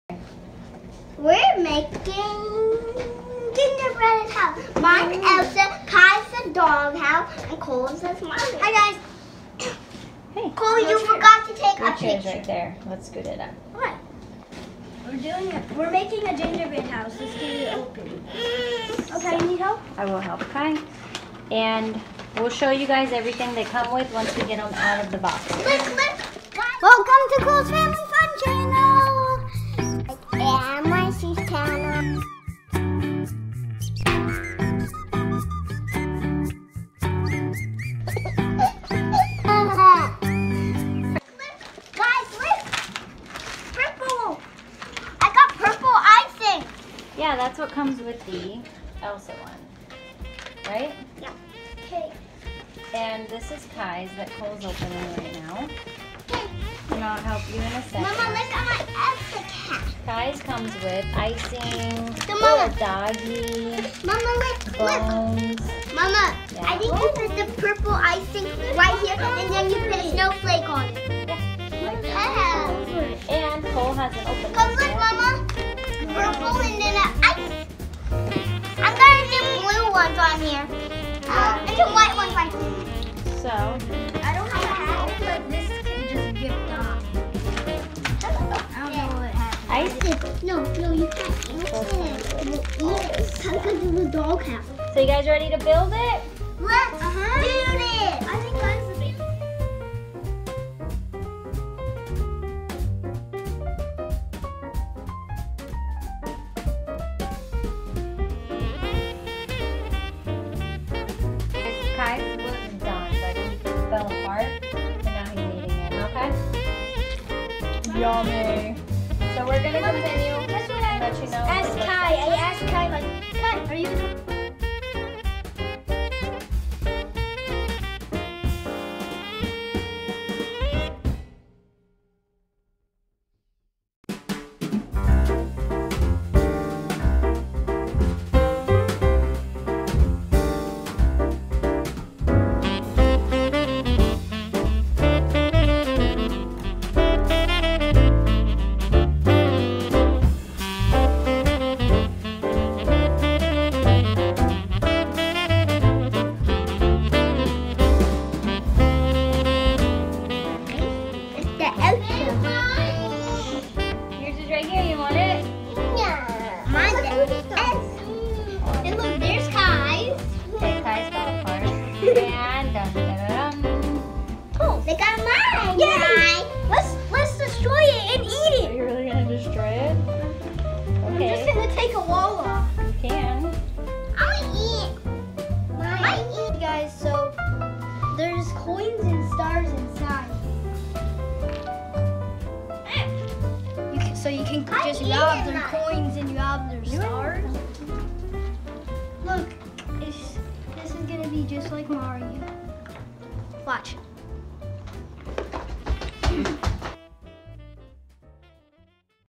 Super Mario Castle And Frozen 2 Gingerbread Houses
Hey guys ! Today cole and marci are making supermario and frozen 2 gingerbread houses. Their little brother had a dog one ,but it was a fail lol but he had fun making it.  We hope you enjoy this video and wish everyone a merry christmas